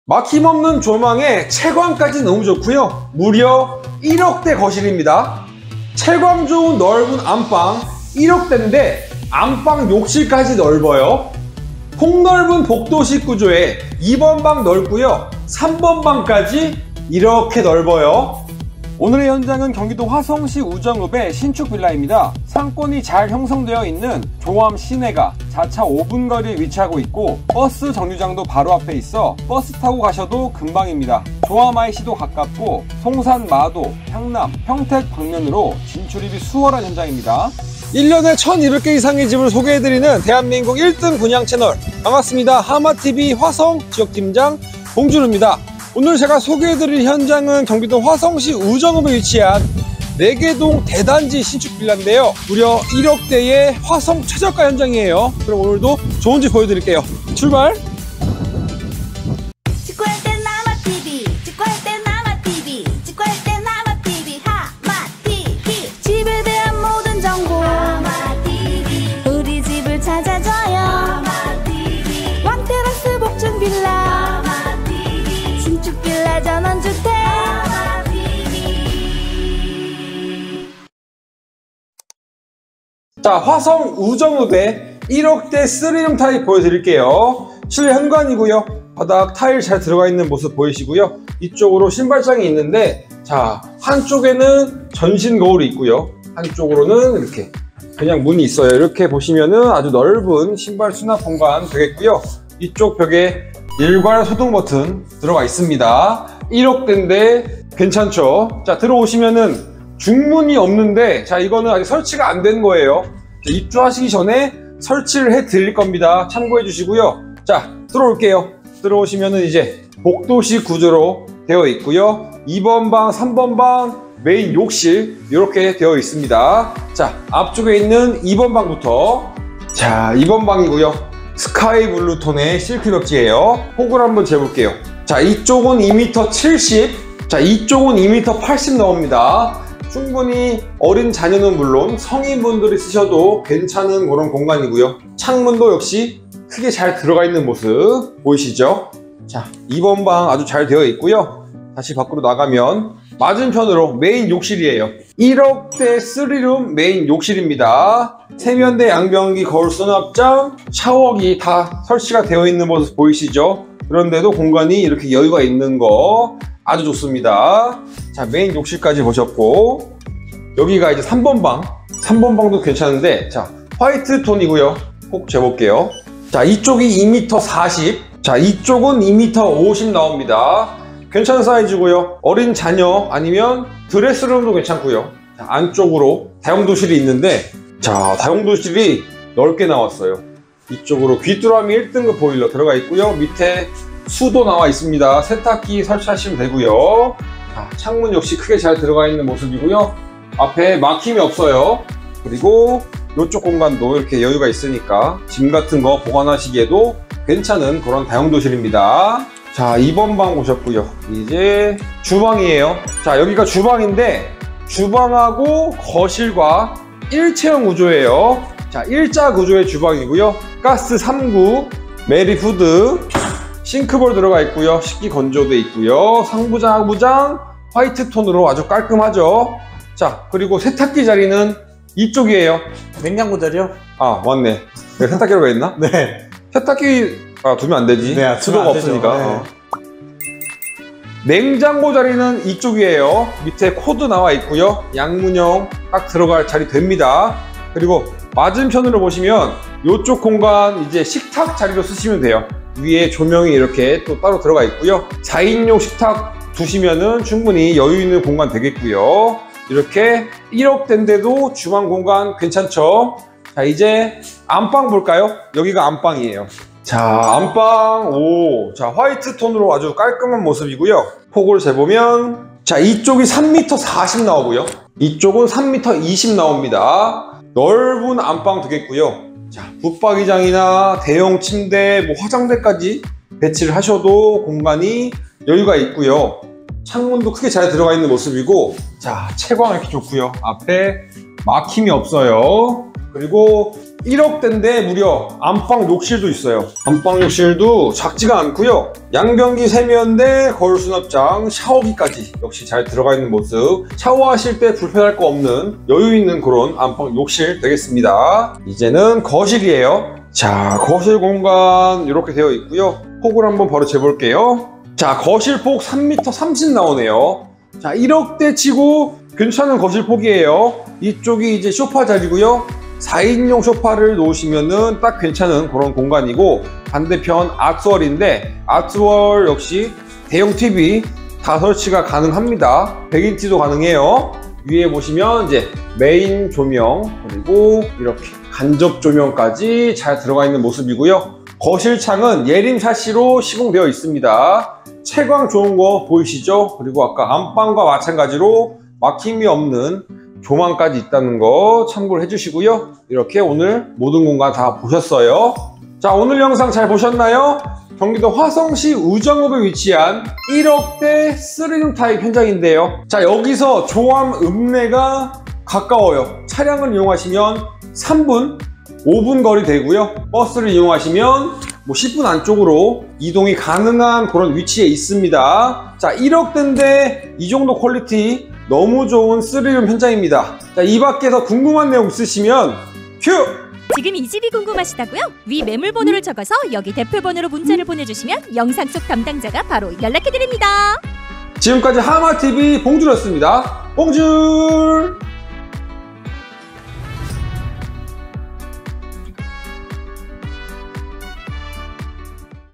[화성신축빌라] 우정읍에 최개할인! 최저 분양가 등장! 아마 전국에서 가장 저렴할꺼예요 /5416
안녕하세요^^

구독자 여러분의 안전한 주택분양 길잡이
하마tv의 봉쥬르 입니다..^^

저희 하마티비의 꿀매물들을
가장 빨리 보고 싶은 분들은
눌러 주시기 바랍니다

◐위치◐

경기도 화성시 우정읍

◐타입◐

방3 기준층

◐특징◐

4개동 44세대

조암읍내 차량5분

무입주 가능!

기아자동차 화성공장, 우정공업단지 등 출퇴근 용이

1층 넓은 자주식 주차100%이상 가능

1억중반 3룸

엘리베이터, CCTV, 친환경자재

- 봉쥬르의 말 -

1억중반 대단지

방3개 저렴한 현장

조건부 무입주 가능!

★분양가★

방3 기준층 1억중반

★실입주금★

조건부 무입주

언제나 좋은 현장만 찾아서 보여드리는
하마TV 봉쥬르 였습니다~